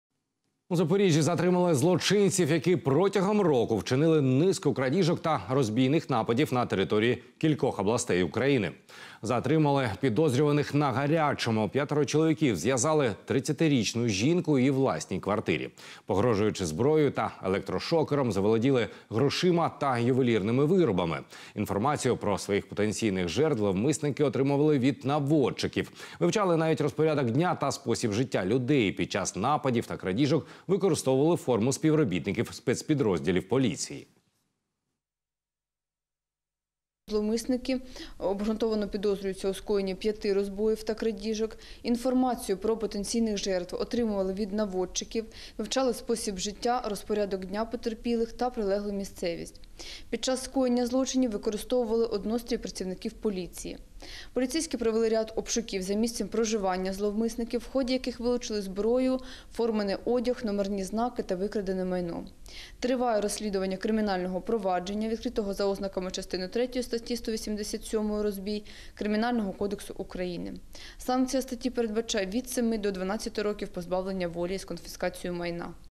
Затримали
У Запоріжжі затримано злочинну групу, яка протягом року вчинила низку крадіжок та розбійних нападів на території кількох областей України.  Затримали підозрюваних на гарячому. П'ятеро чоловіків зв'язали 31-річну жінку у її власній квартирі. Погрожуючи зброєю та електрошокером, заволоділи грошима та ювелірними виробами.